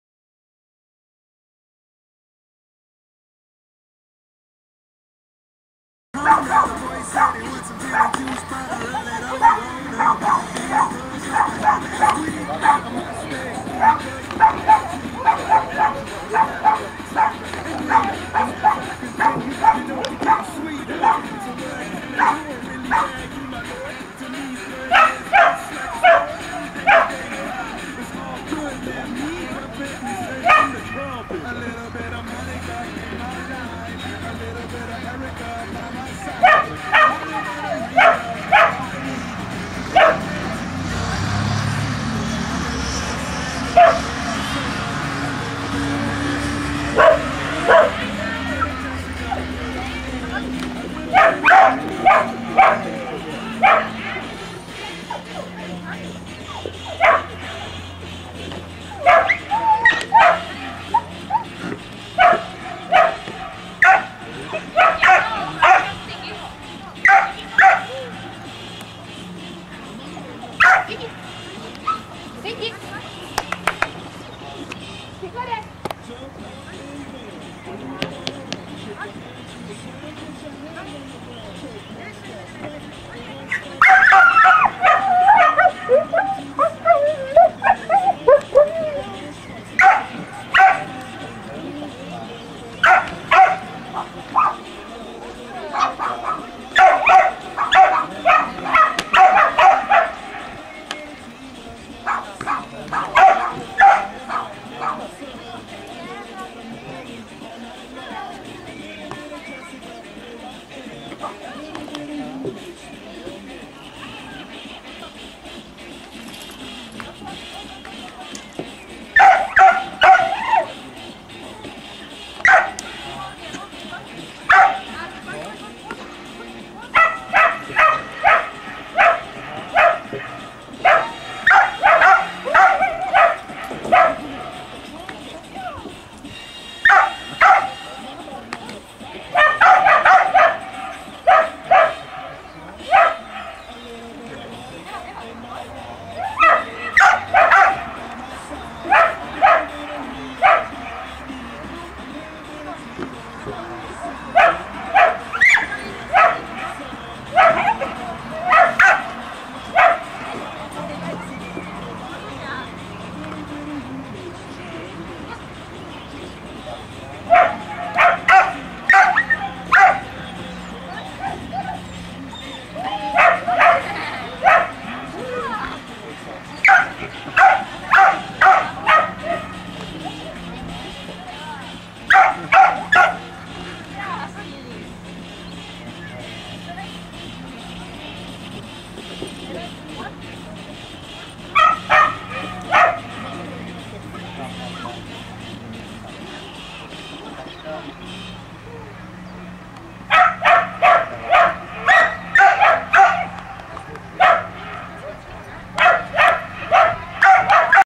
Ukázka dogfrisbee Arlin + Kony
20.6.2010 ukázka dogfrisbee při závodech agility v Měchenicích.